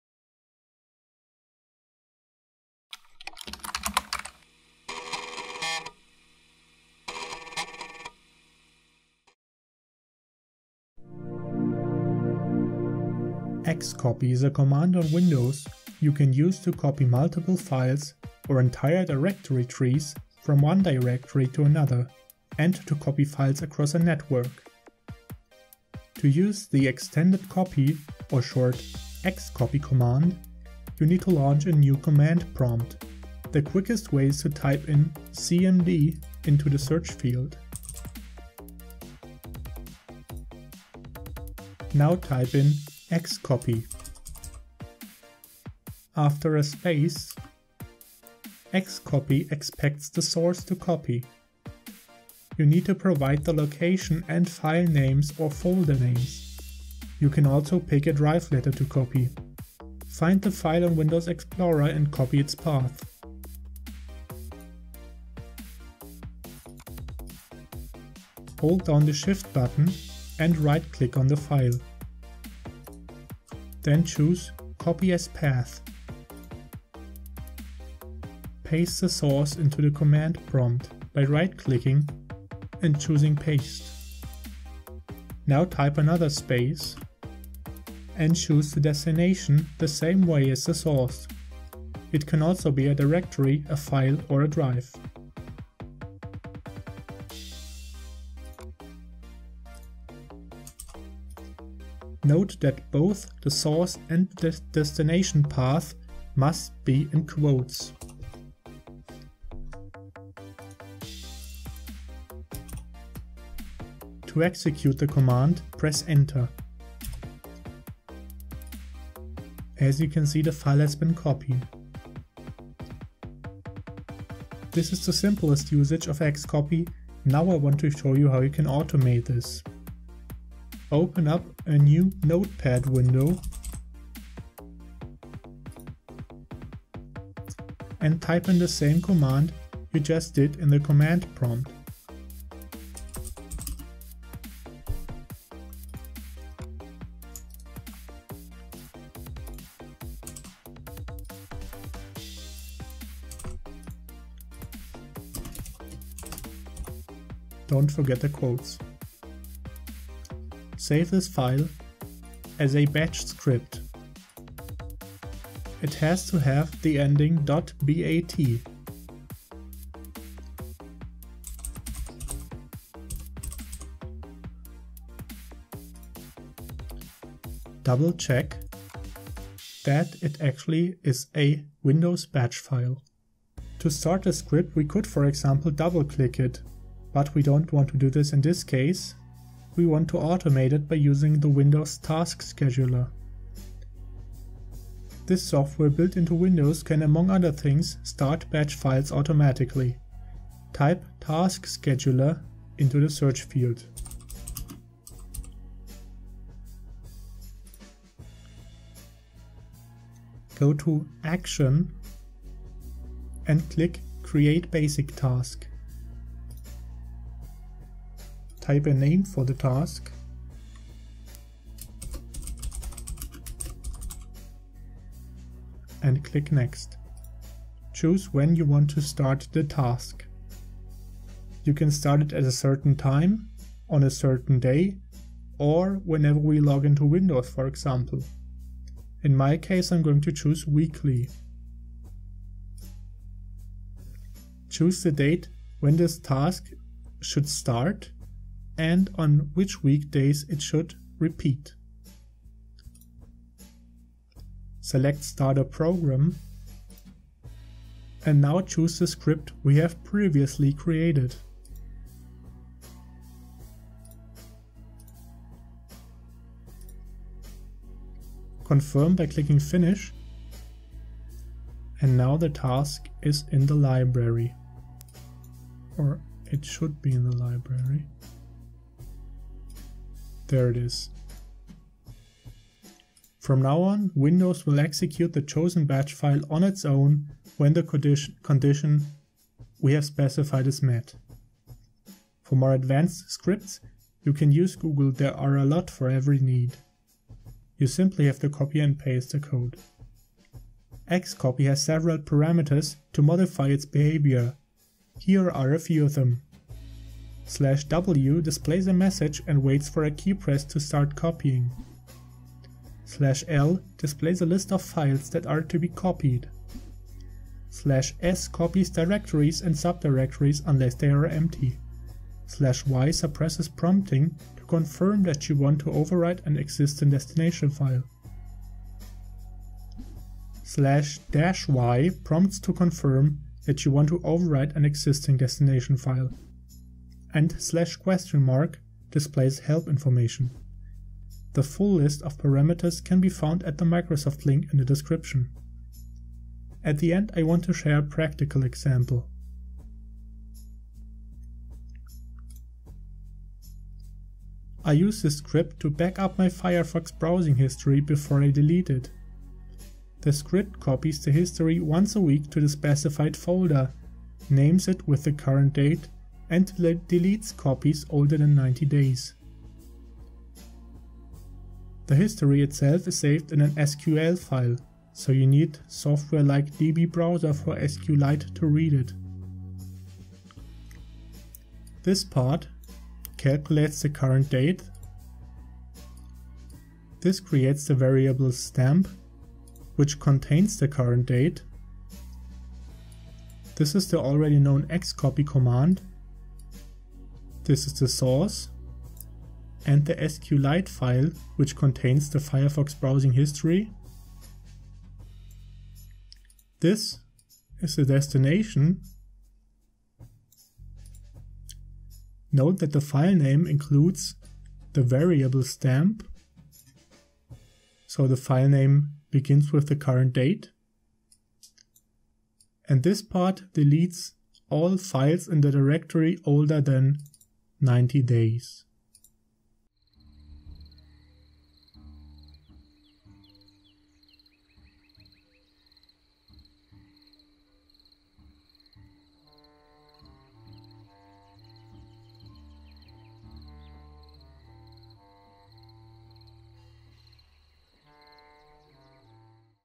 The Easy Guide To Automatic Backup in Windows with XCOPY
Learn how to use XCOPY, a built-in copy tool for Windows. This tutorial shows the basics of XCOPY and an example for how to backup the Firefox Browser History on a weekly basis, name the file with the current date and delete any copies older than 90 days. No need for third-party software!

You want to support me and my videos? Any amount is appreciated!